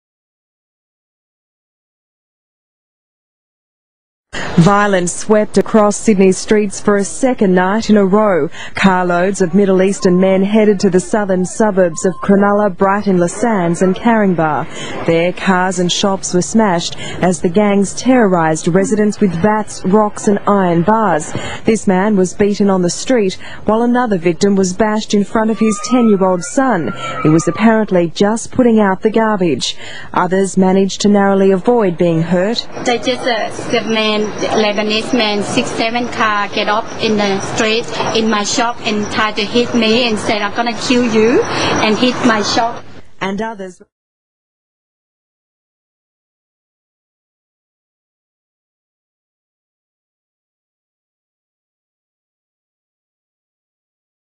Dissappearing news stories
now when everyone is pushing multiculturaism  and telling us ghetto's are wonderful places

a reminder of the facts ..

for those falling for the PR white wash  such as today news brings ,  just todays remember

i offer this short clip as a reminder  of what this brings  with some groups
many races have come here and been part of this nation for years , but they assimilated , many dont and only wish dominance and control , we need a monoculral society of many races to ensure our land stays as free as is always has for ALL people of whatever background , but Aussies all !